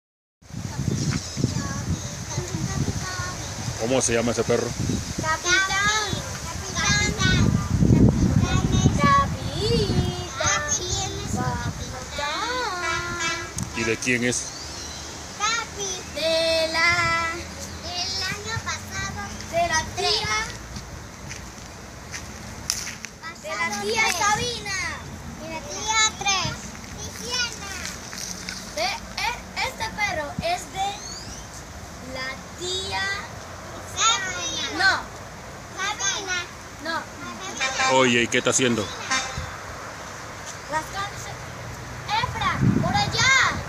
Casís: Recuerdos Familiares Parte 309/ Capitán el Perro de Coclé.
Las hermanitas Casís, presentan al mundo entero a Capitán.